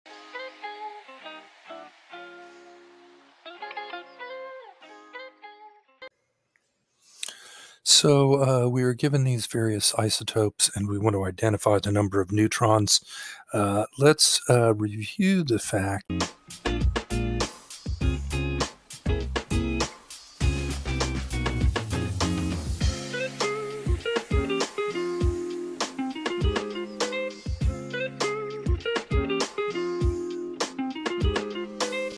The following radioactive isotopes are used in medicine for imaging organs, studying blood circulat…
The following radioactive isotopes are used in medicine for imaging organs, studying blood circulation, treating cancer, and so on. Give the number of neutrons present in each isotope: ^198 Au,^47 Ca,^60 Co,^18 F,^125 I,^131 I,^42 K,^43 K,^24 Na,^32 P,^85 Sr,^99 Tc.

discover the power of radioisotopes: applications and benefits, the fundamentals of atoms and basic elements